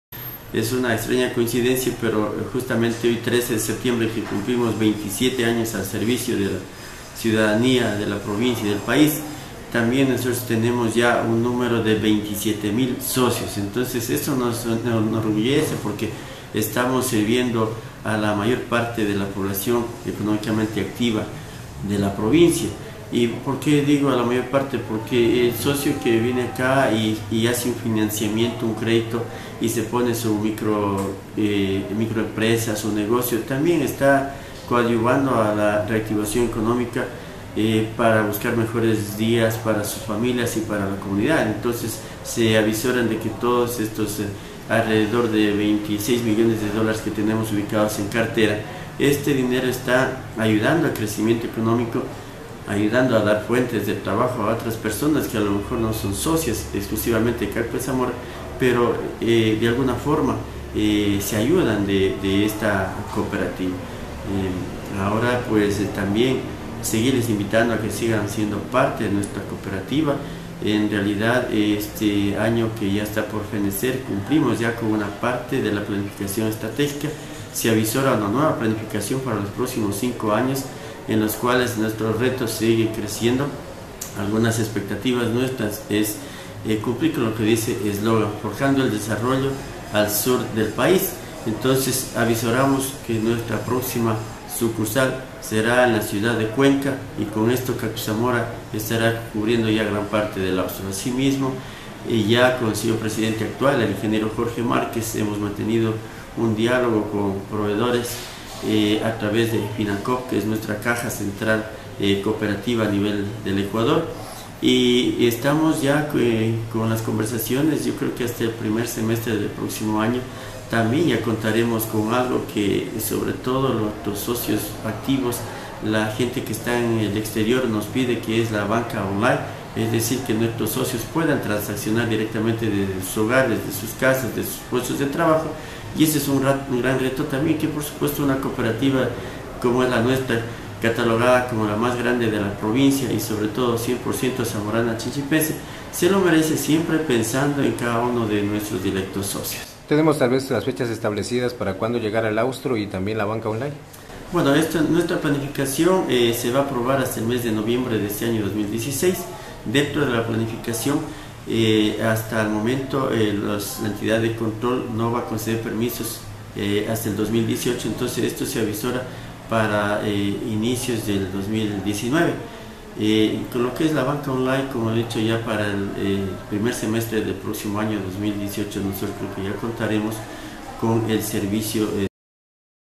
Cacpe Zamora celebró 27 años al servicio de Zamora Chinchipe y el País
Nixon Gonzalez, Gerente de CACPE Zamora manifestó "Hoy 13 de septiembre cumplimos 27 años al servicio de la ciudadanía de la provincia y del país".